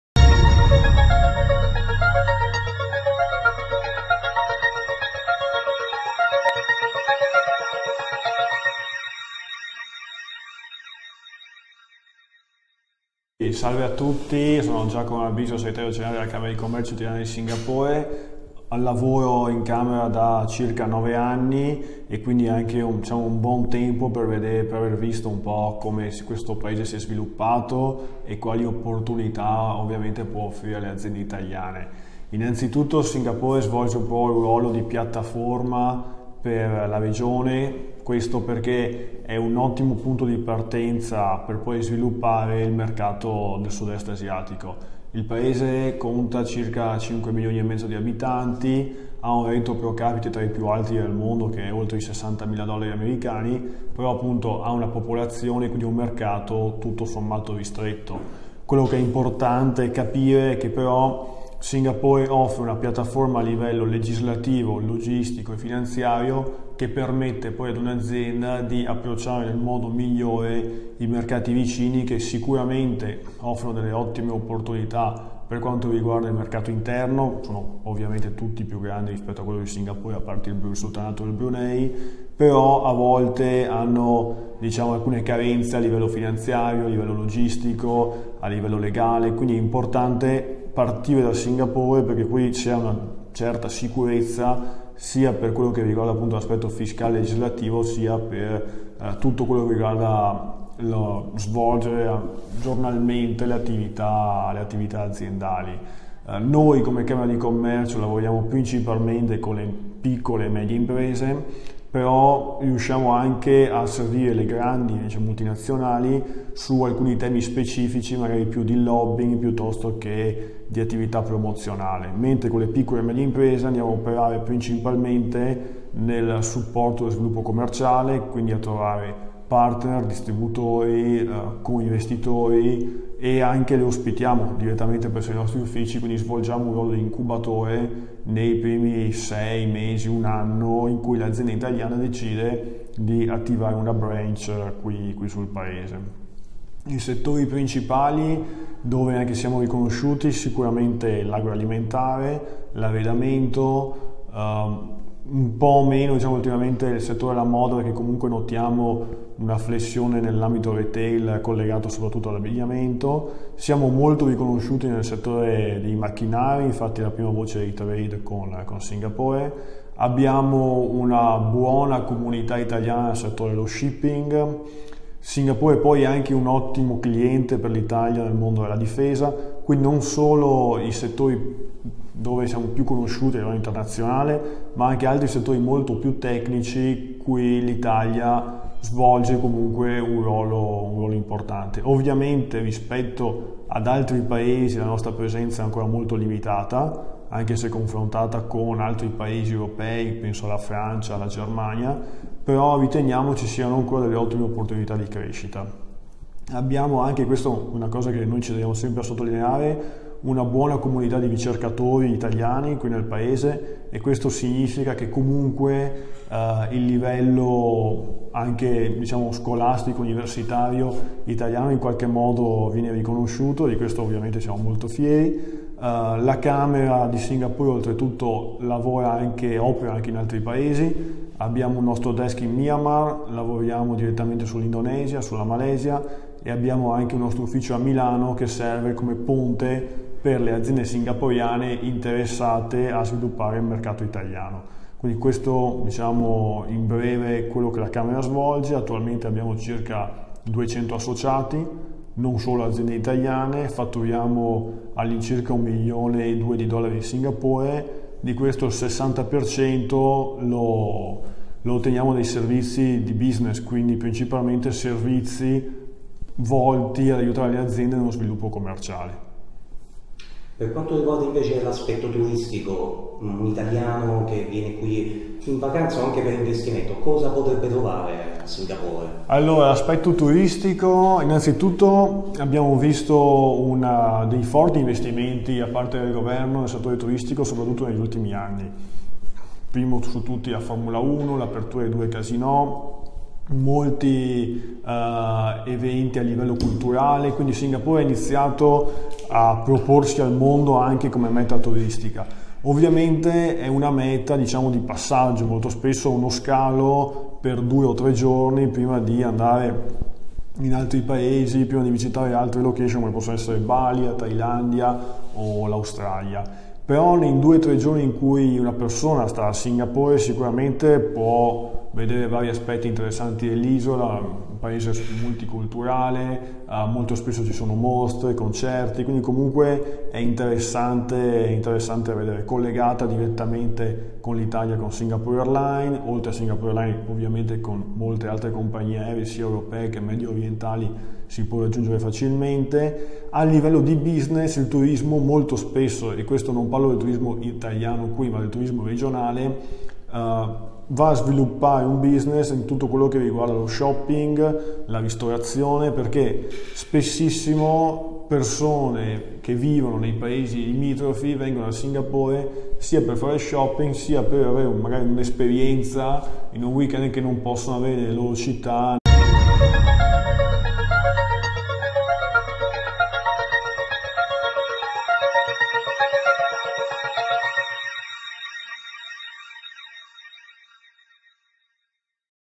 Intervista a Giacomo Marabiso - Segretario Generale della Camera di Commercio italiana a Singapore
Giacomo Marabiso, Segretario Generale della Camera di Commercio italiana a Singapore, intervistato per Eurasia News dal nostro corrispondente Marco Iaconetti.